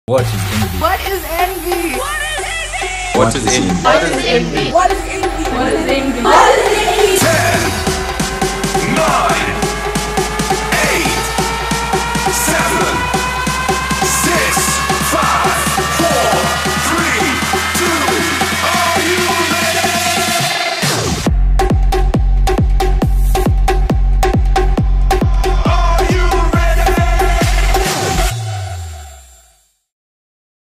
Club Night Promotional Video
ENVY- hosted by @michelleandairborne in Harare Zimbabwe is a pop up nightclub event